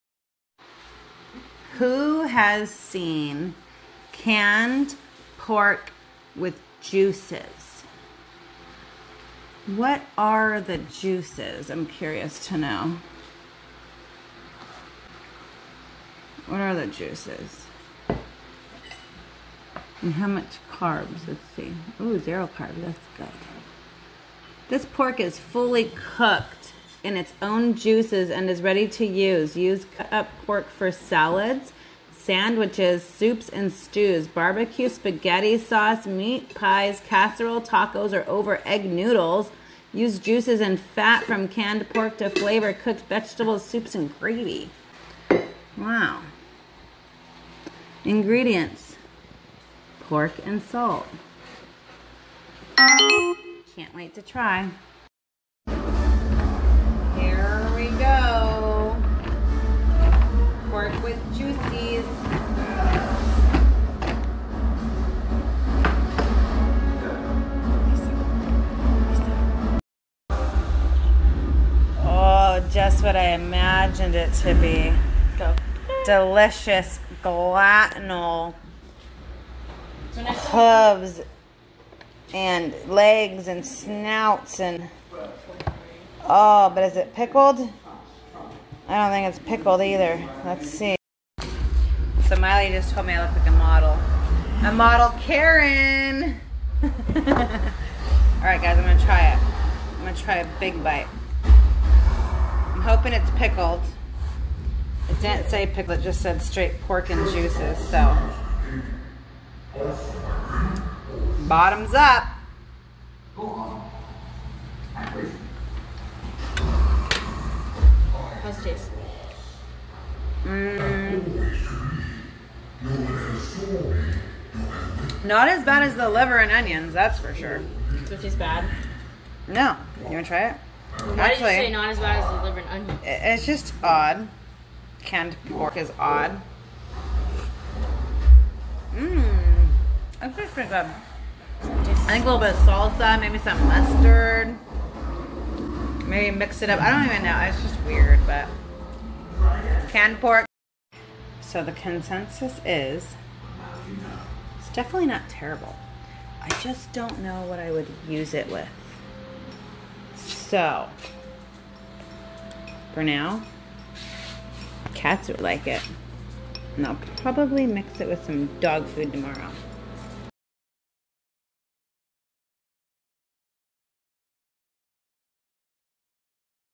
Canned pork, Mmm Mmm Good!
Tasting this little Gem for the first time.